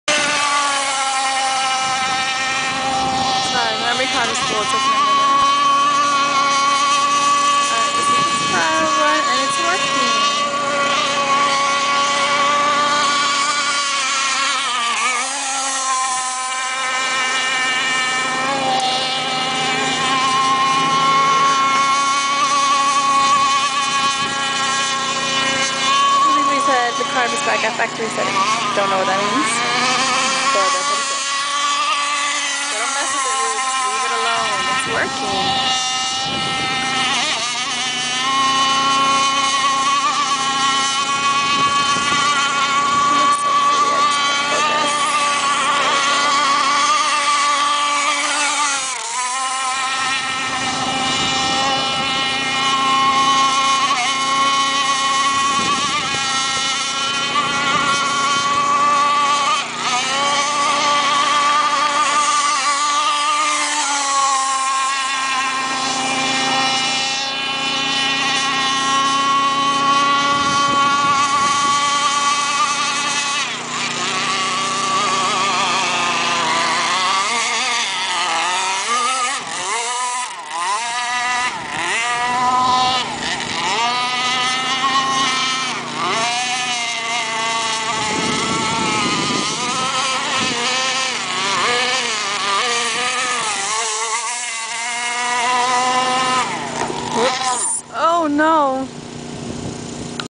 Melbourne Open water testing 6-10-11.MPG
This is how rough the water was when we got to the lake for open water testing.